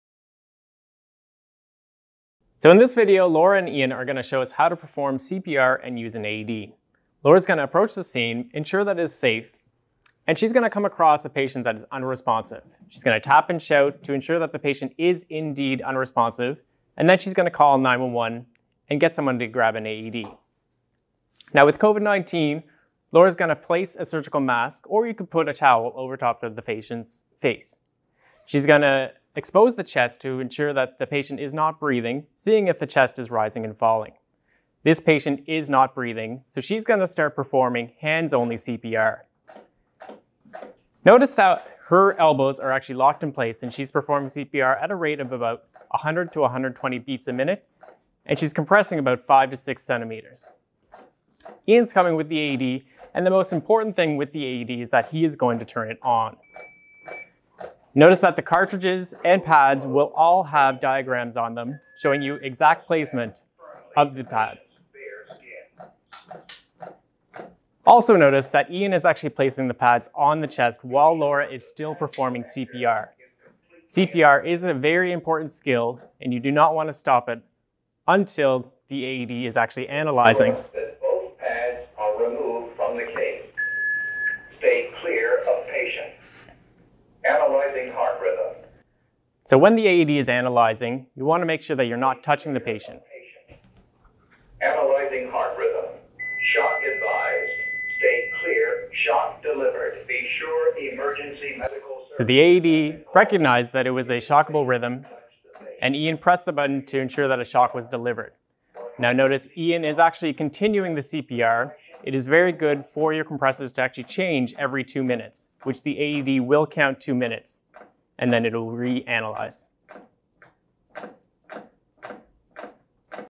How to perform CPR
Paramedics Laura and Ian show how to perform hands-only CPR and use an AED.